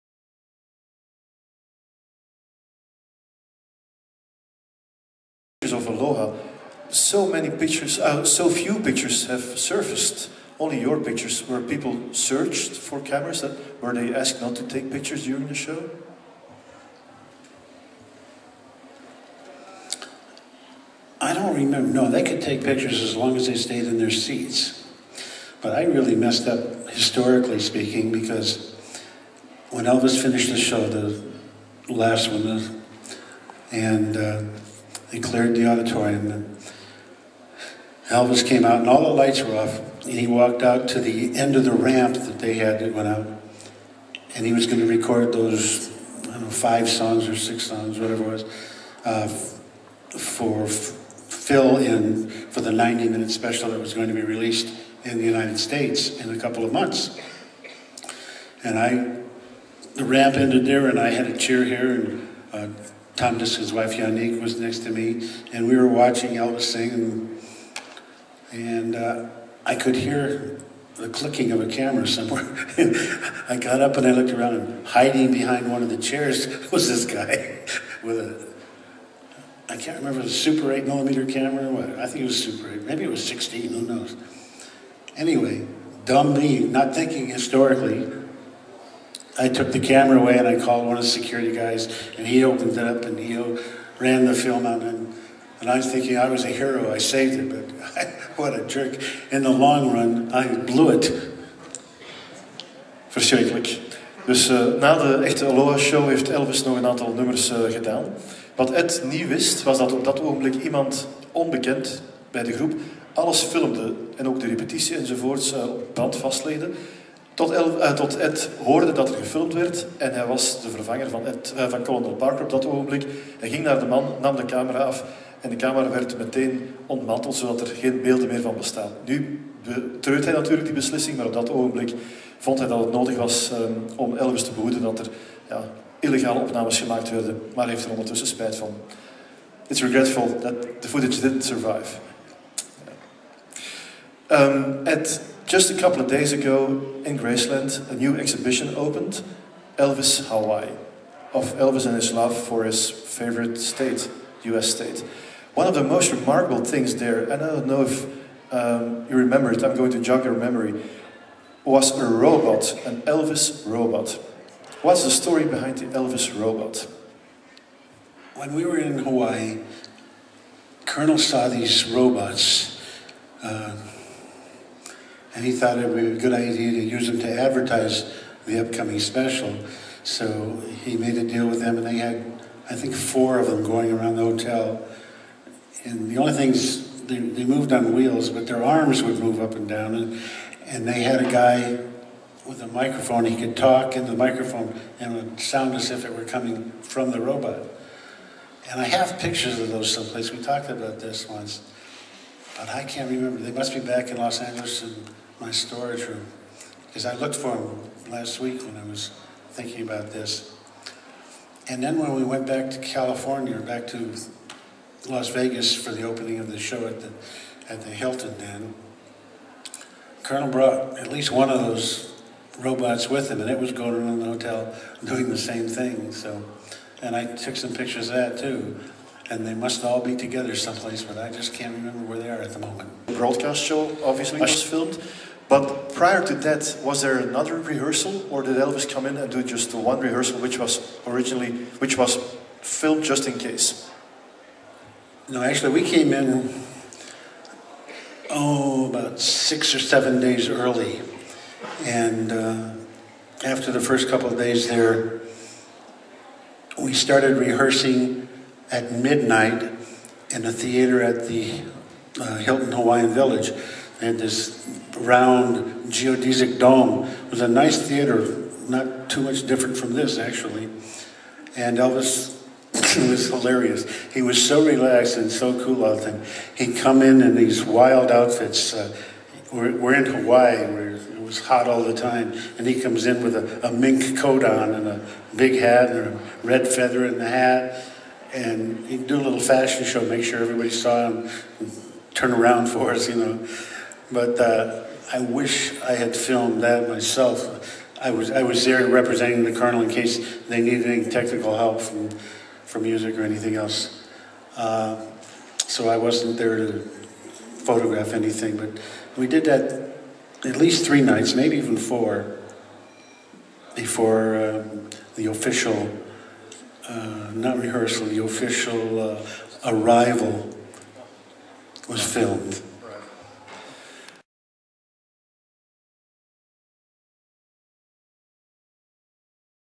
Peter Verbruggen talks with Ed Bonja
Peter Verbruggen was journalist bij het Belgische dagblad "Het Laatste Nieuws" en oprichter / voorzitter van de Elvismatters Fanclub.
Zijn passie en bevlogenheid voor Elvis en zijn grote betrokkenheid bij Elvismatters en haar leden, maakten hem tot een zeer geziene en geliefde voorzitter.
Peter overleed geheel onverwachts op 15 maart 2015 aan de gevolgen van complicaties bij een griep.